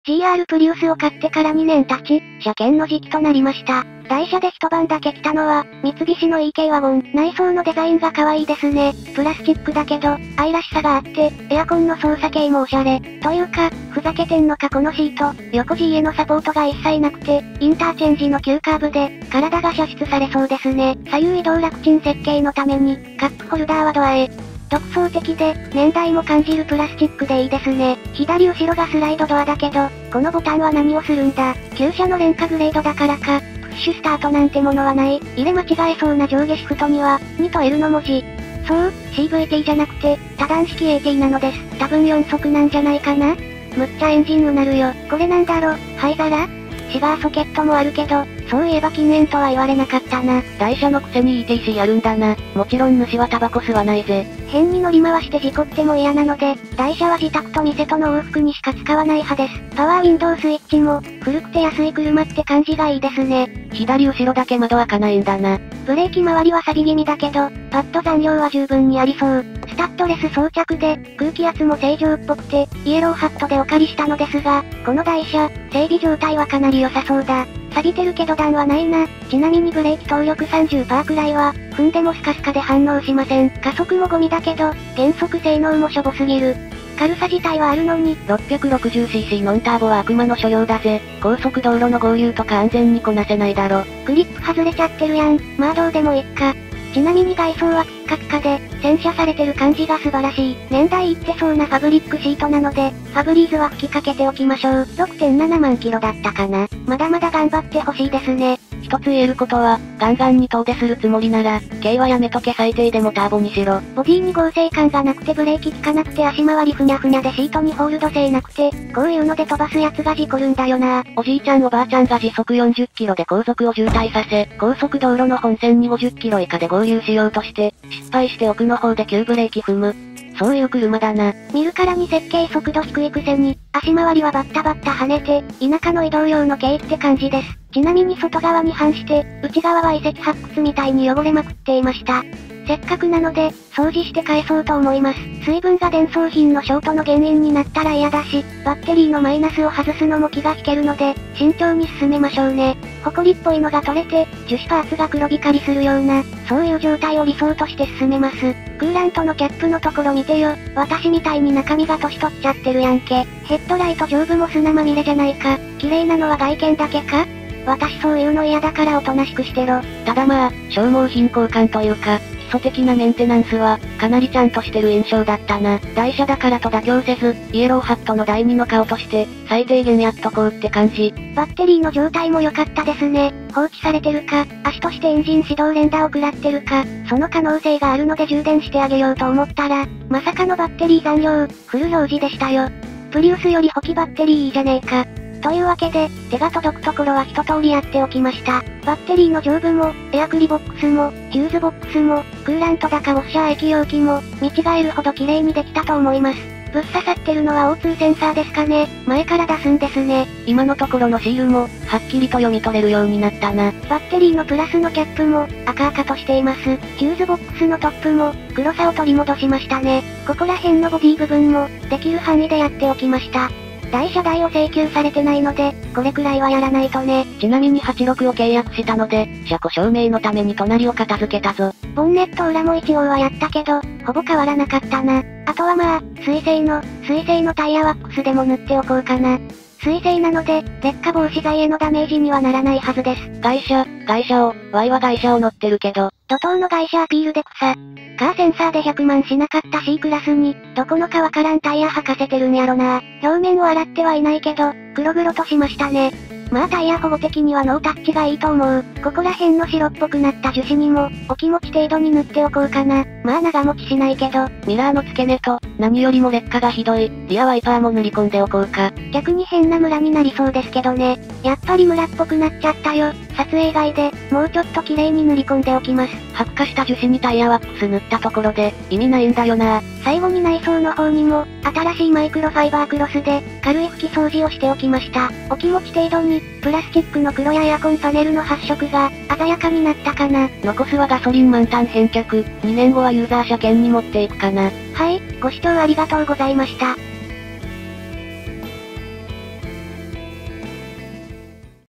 車検の代車のエンジンルームが遺跡発掘現場だった‼ 安全に洗車する。
イエローハットに車検を出したら、代車でエンジンルーム汚れまくりなekワゴンが来ました。
エンジンフォームクリーナーと、マイクロファイバークロスを使って、可能な限り綺麗にして返そうと思います。